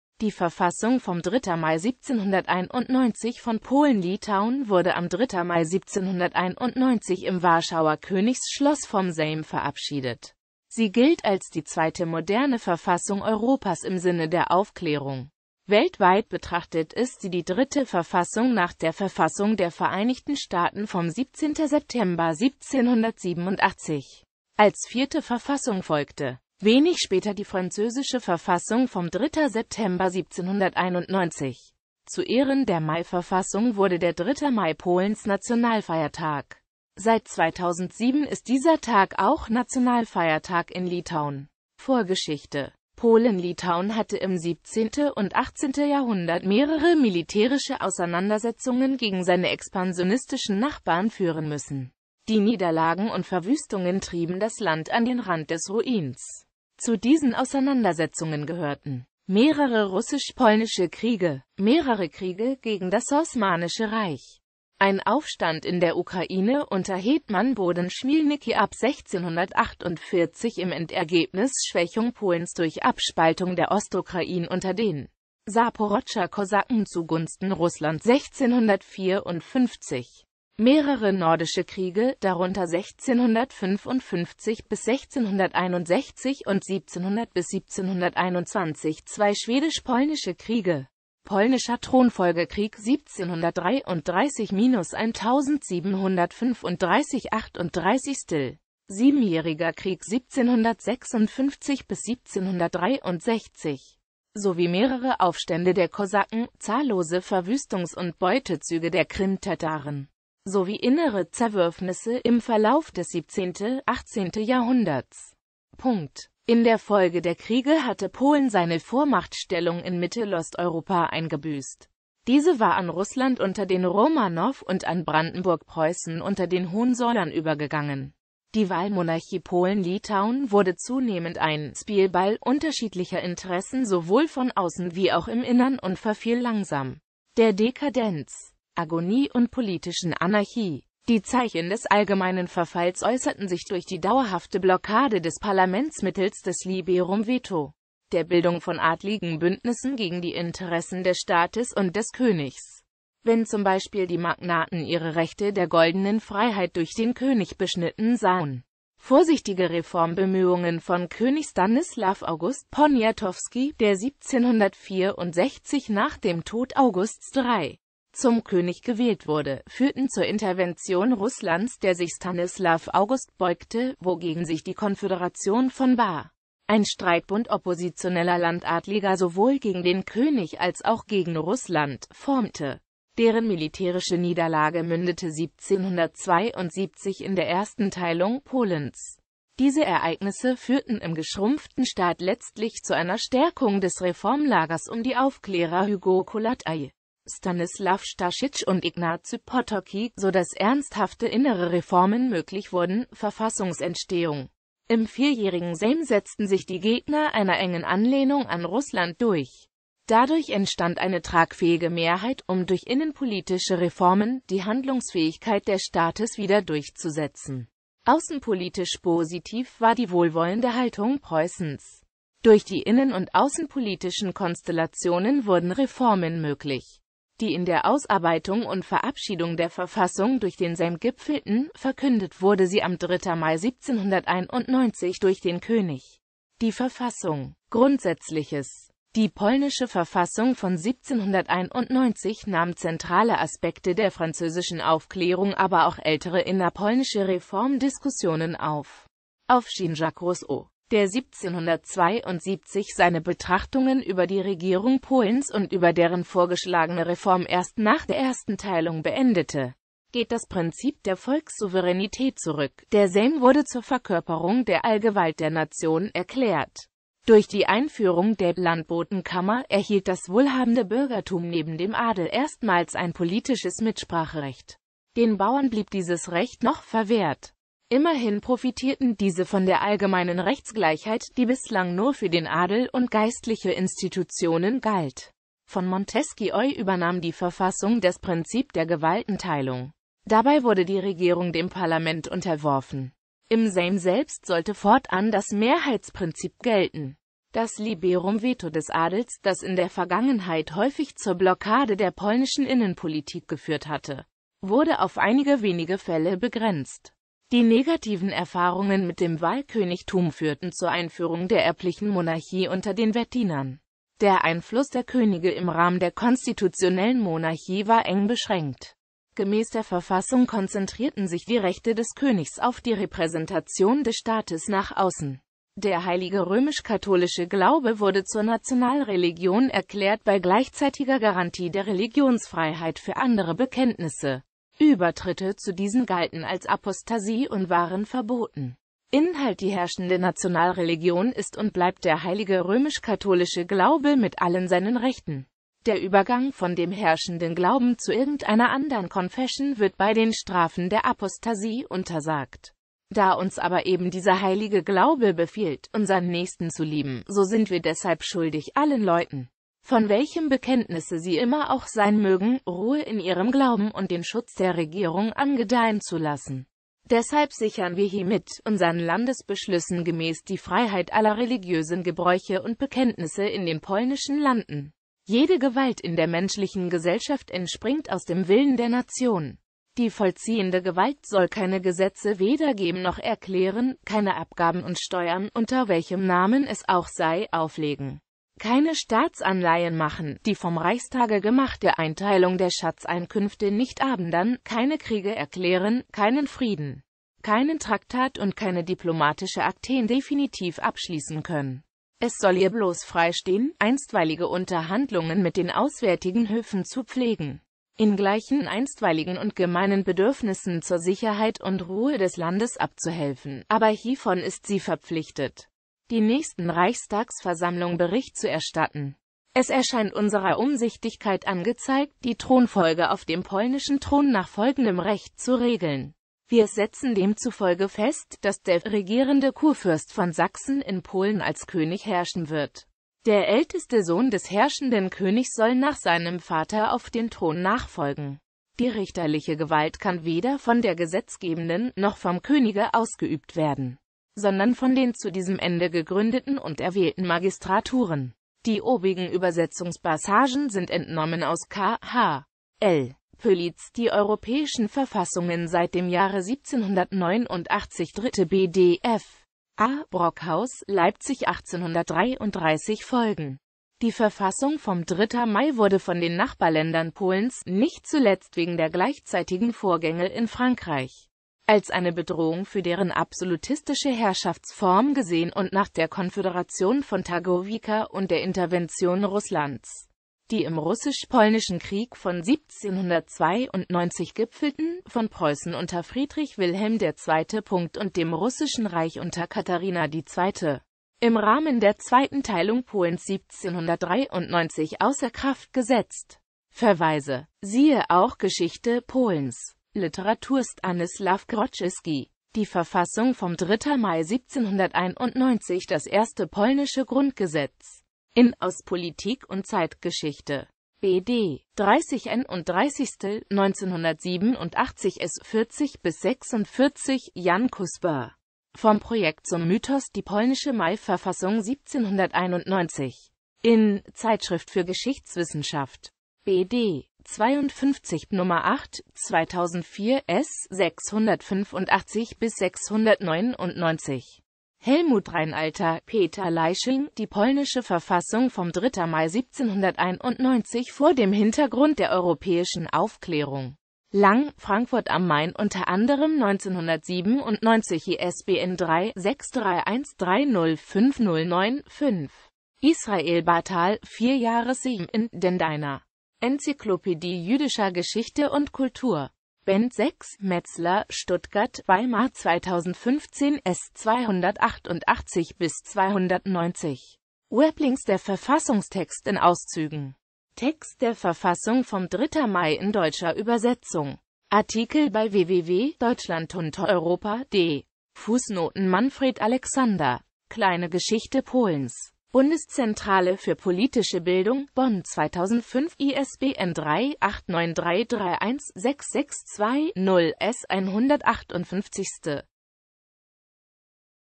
Verfassung vom 3. Mai 1791
Vielen Dank für Ihre Unterstützung:

Die Verfassung vom 3.Mai 1791 von Polen-Litauen wurde am 3.Mai 1791 im Warschauer Königsschloss vom Sejm verabschiedet.Sie gilt als die zweite moderne Verfassung Europas im Sinne der Aufklärung .

Bild-Copyright-Informationen
Urheber Info: User:Mathiasrex Maciej Szczepańczyk, based on layers of User:Halibutt